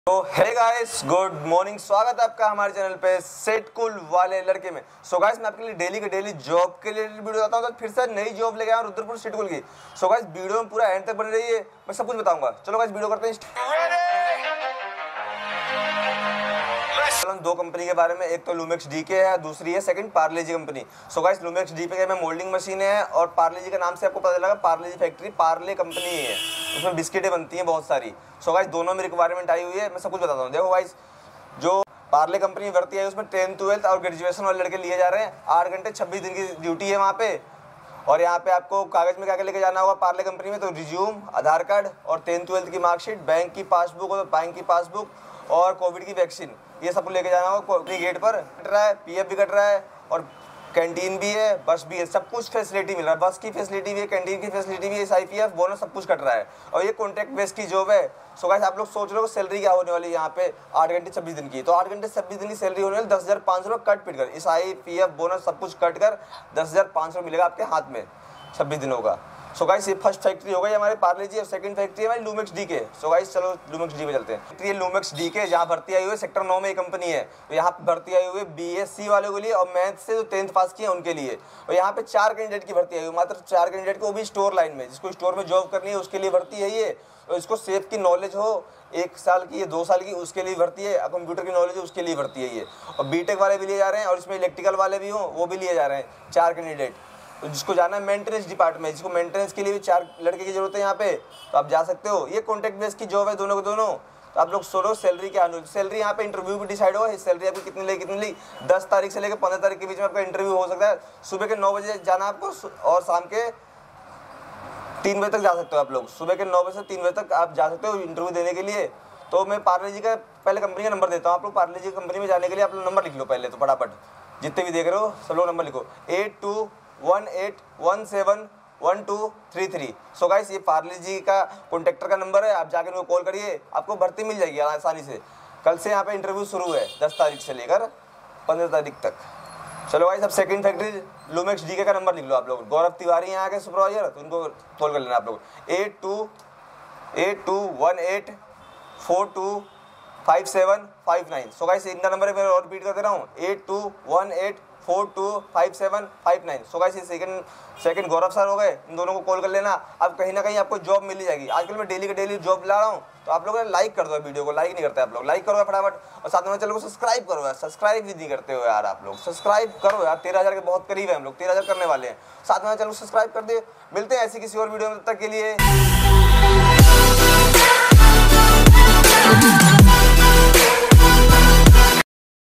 TOP 2 FACTORY VACANCY 20 CANDIDATE RUDRAPUR SIDCUL | SIDCUL WALA LADKA. | 10th pass campny base job
first factory 1

*Urgent Requirement*

*Only boy,s*

*Qualification - 10th,12th, graduation*

*Salary - 10500 8hr26 days OT 120/hr*

*Company name - parle sec -01*

*PF, ESIC, Bonus, Canteen,Bus*

*Interview date & time _ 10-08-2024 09AM*

*Call and send resume*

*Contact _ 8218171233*

second factory 2

Urgent Requirement
Apprentice Trainees NAPS/NATS

Company Name: Lumax DK, Sector 9, Sidcul, Rudrapur

Qualification: B. Sc. Maths 10 Candidate's for
Production

Graduate Only 4 Candidate's (Store)
Experience: 1-2 Year + Computer knowledge/SAP

B. Tech/Dipl. - Electrical/Mech.
(4 Candidate's)
Exp: 1-2 Year Maintenance Department

ITI Molding Machine Maintenance                                                                                                                                                   Experience: 1-2 Year (4 Candidate's)

CIPET 4 Candidate's with 0-2 Year Exp.

Stipend: As Par Interview

Interview Timing - 09:00 AM

Call Or Send Resume
Gourav Tiwari